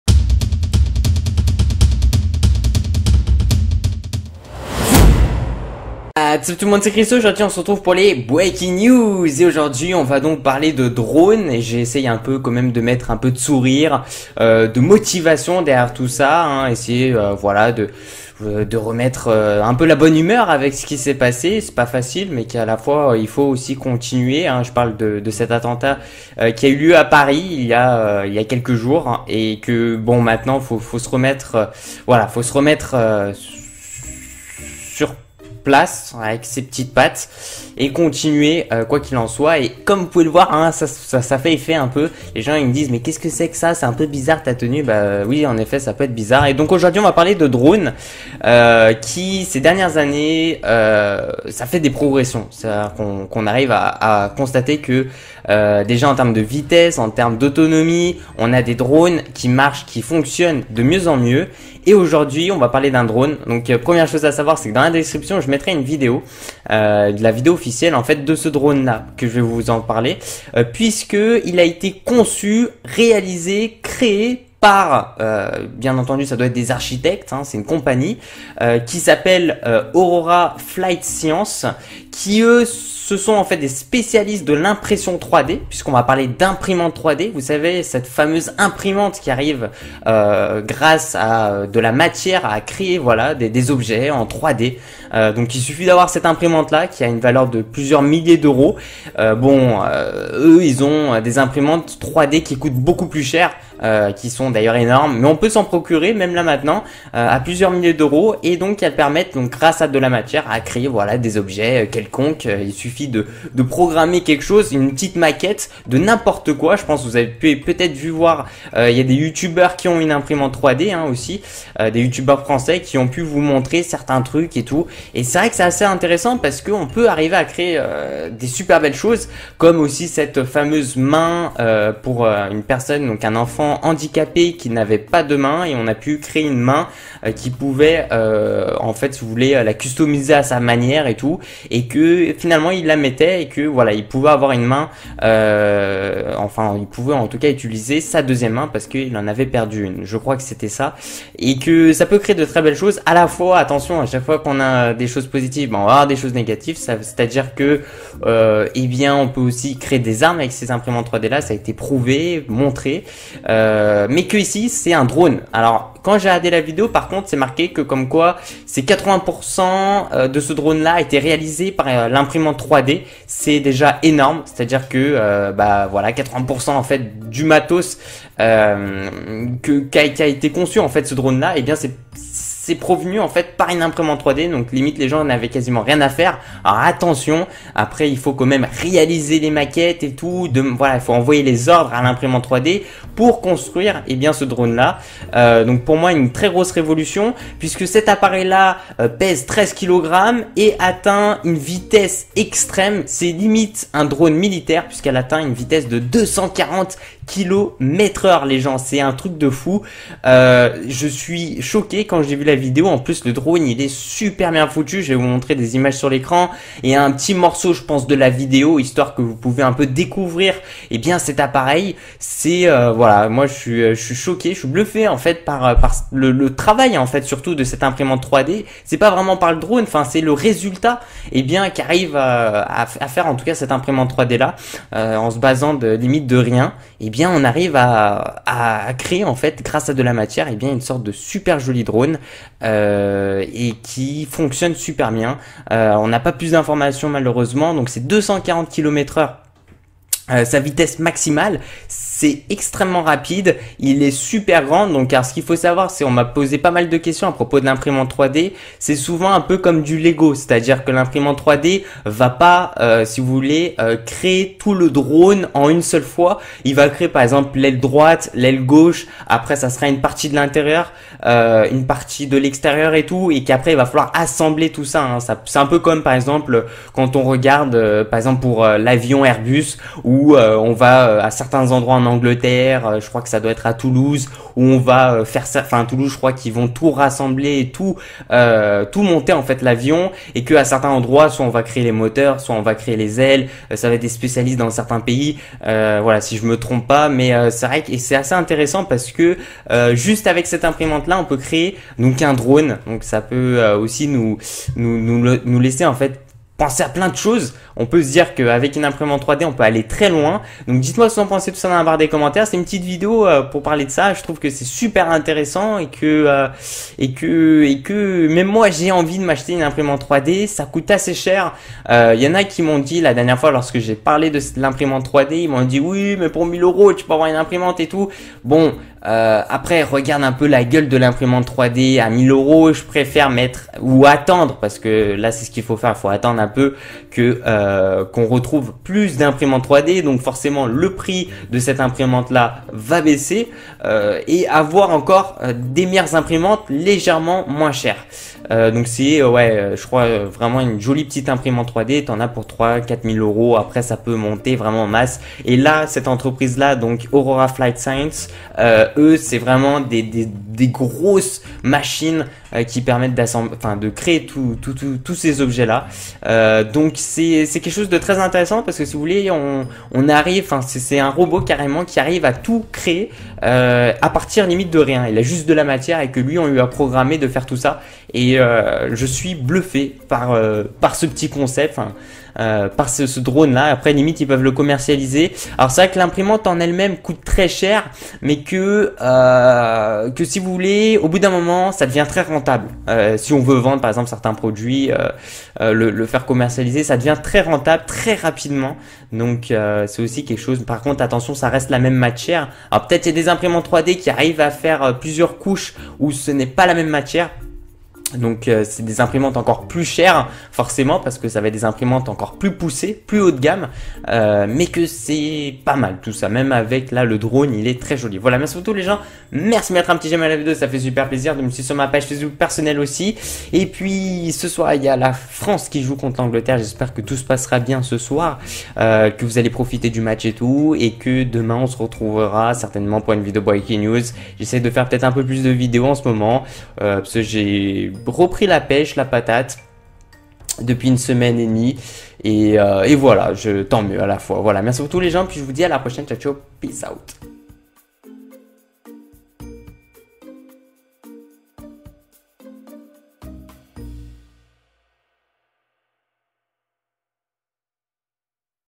IMPRIME EN 3D, CE DRONE à réaction atteint les 240 km/h !
100 j'aimes = 1 vidéo en forme :D

Merci :) !